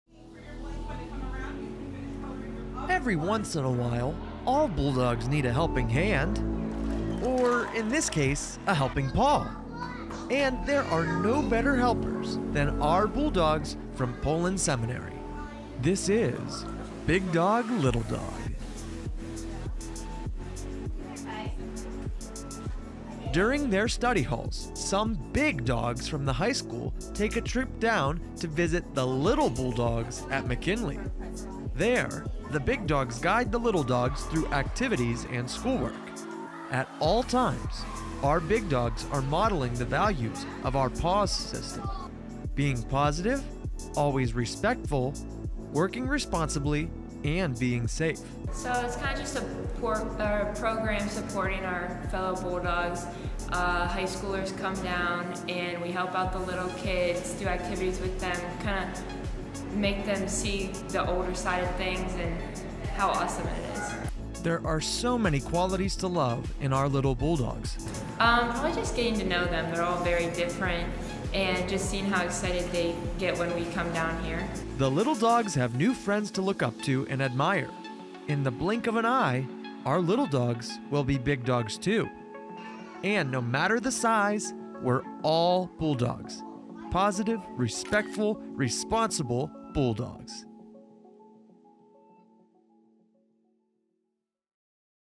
One Dog, Two Dog... Big Dog, Little Dog!
Our little dogs at McKinley made new friends in the BIG DOGS of Poland Seminary High School! Learn about the PAWSitive initiative occurring in our district!